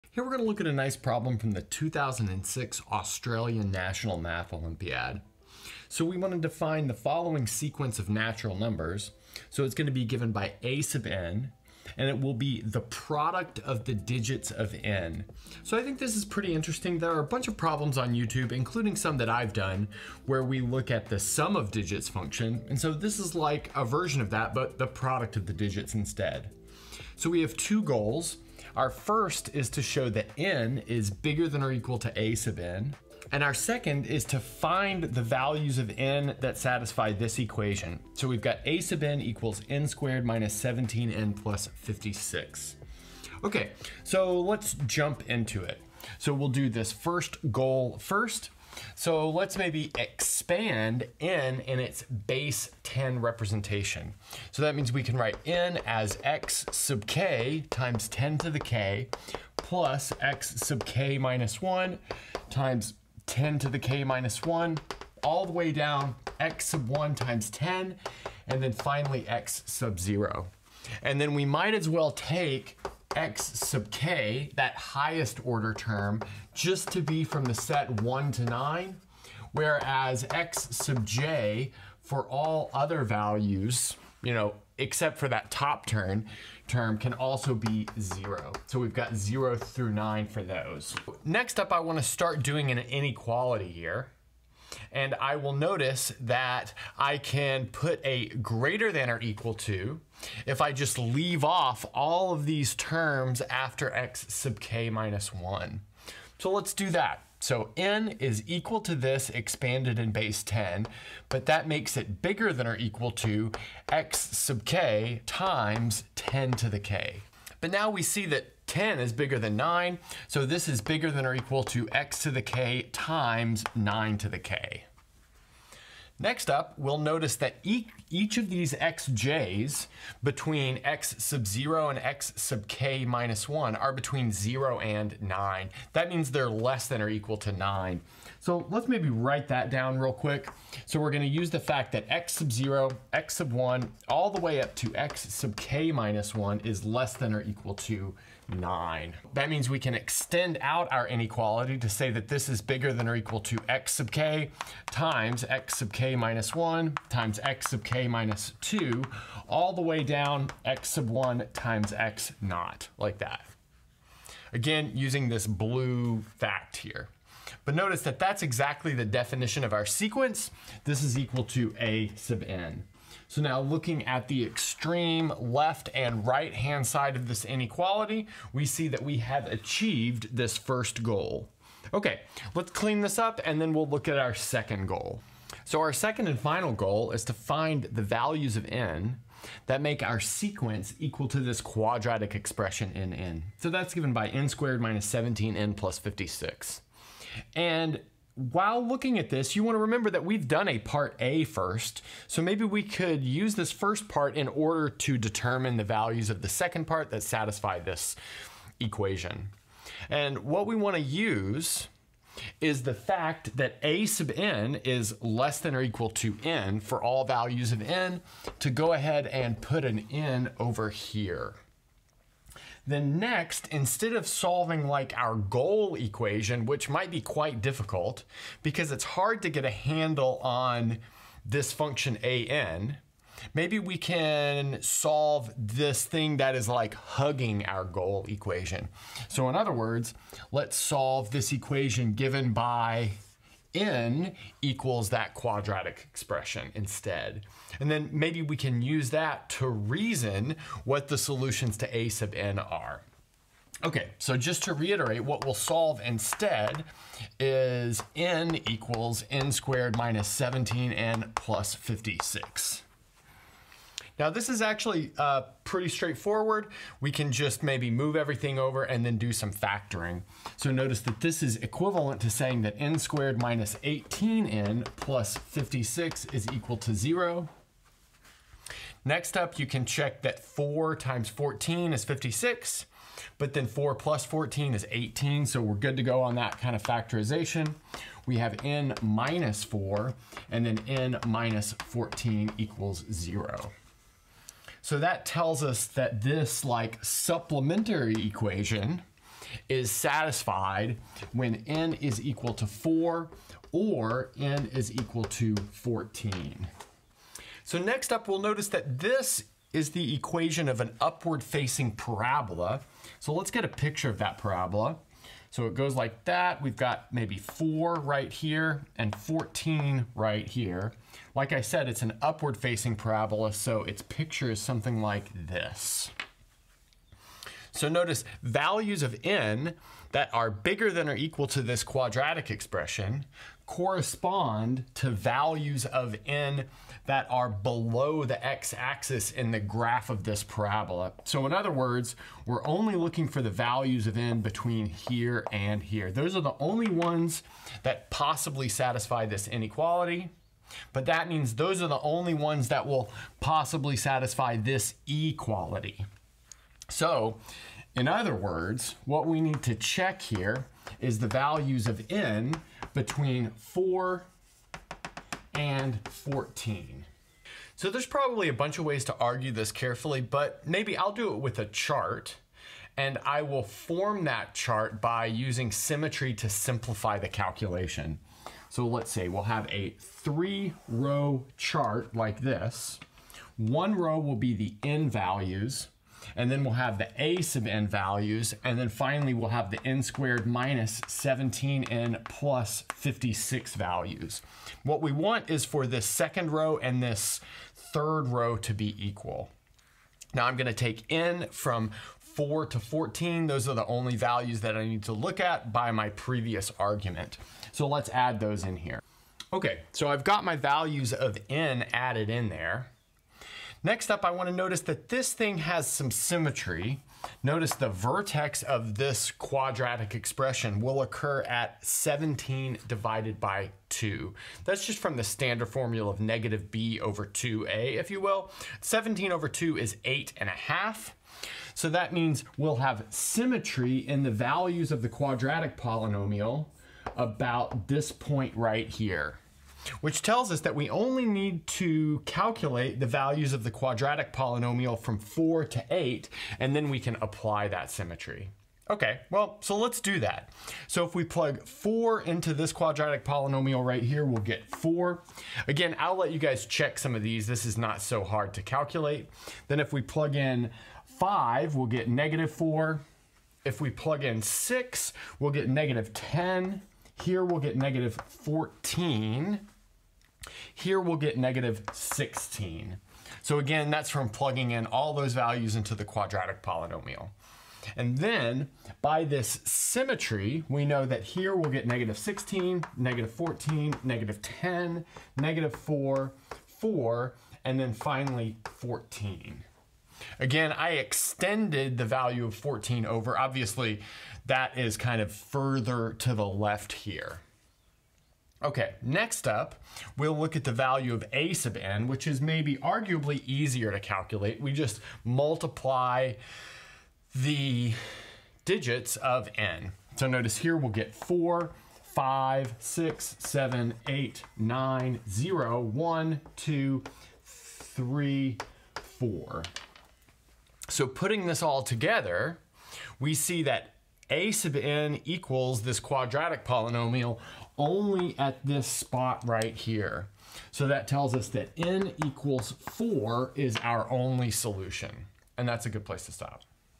From Australia...
We look at a nice problem from the 2006 Australian National Math Olympiad.

Differential Forms: